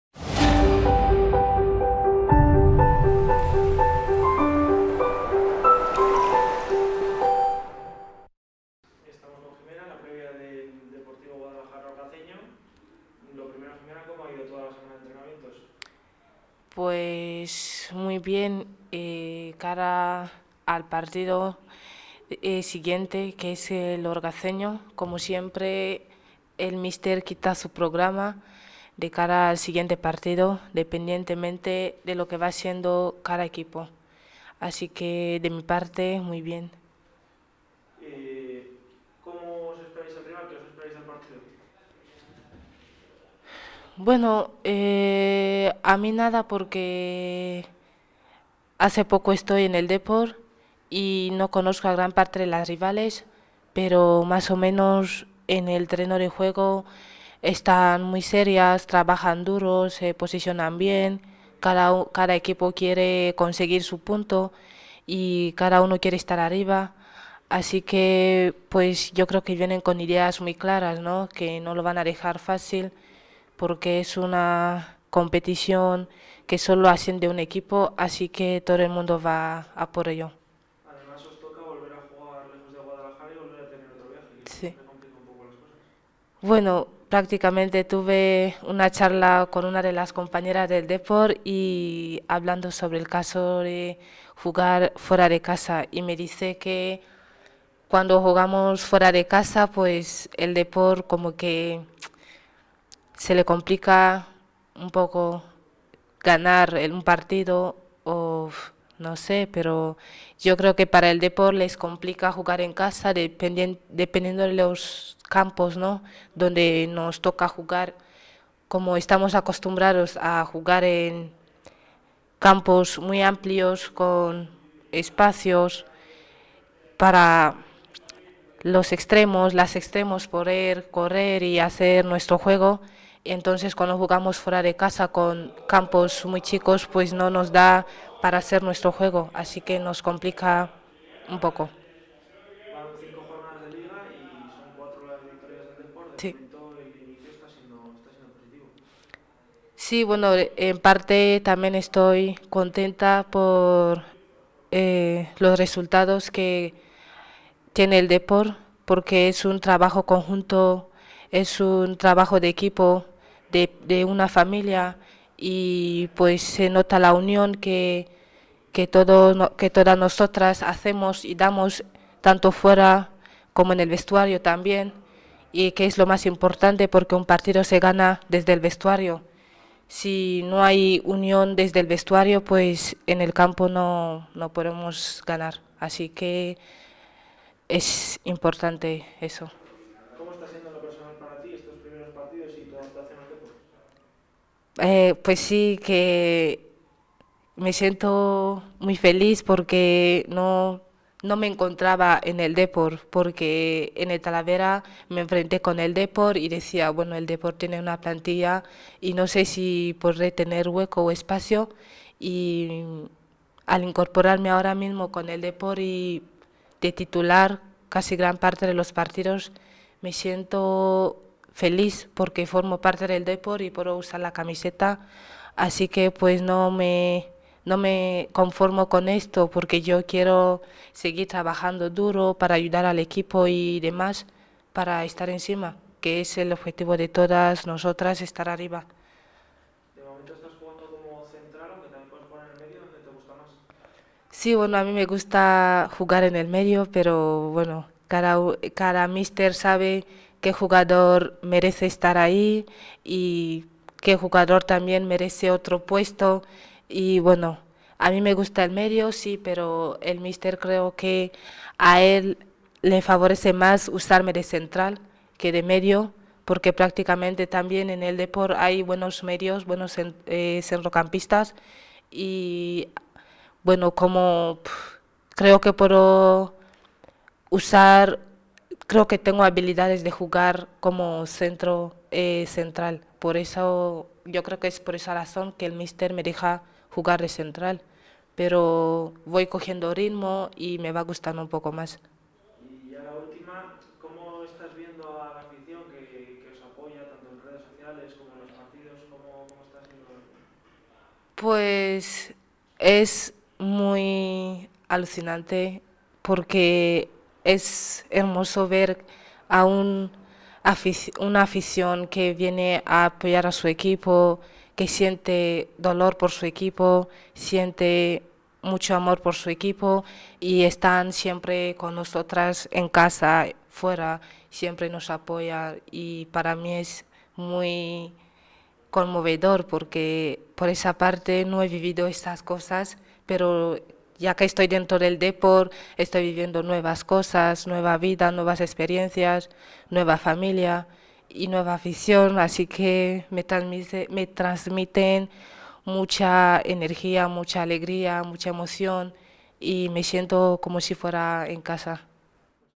Bikie Boko habla en la previa del Orgaceño - Deportivo Guadalajara Femenino
La jugadora africana analizó el desplazamiento que tendrá su equipo este fin de semana